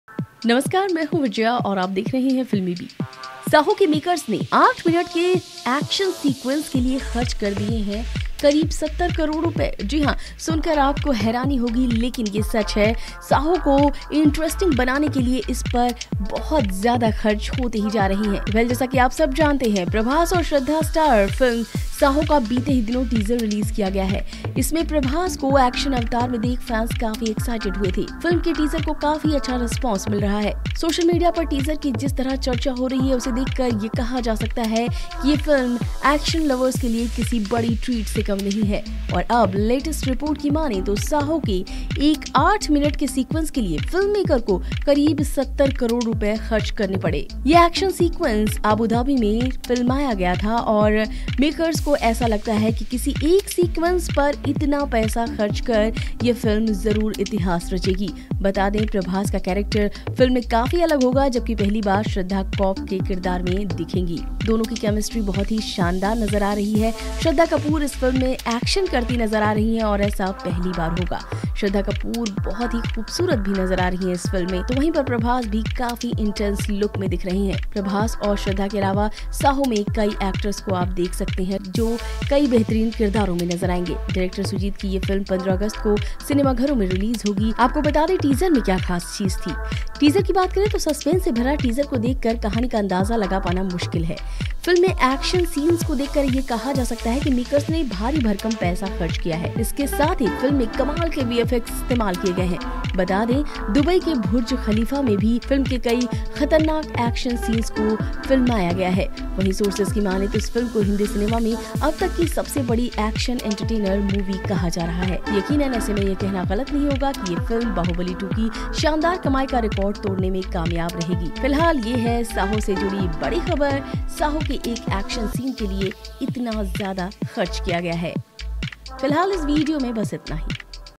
Prabhas' Saaho to feature the most expensive stunt | FilmiBeat
Prabhas' Saaho to feature the most expensive stunt. Baahubali actor Prabhas had shot an eight-minute action sequence for Saaho in Abu Dhabi earlier this year and the production cost of this scene is Rs 70 crore, says cinematographer Madhie.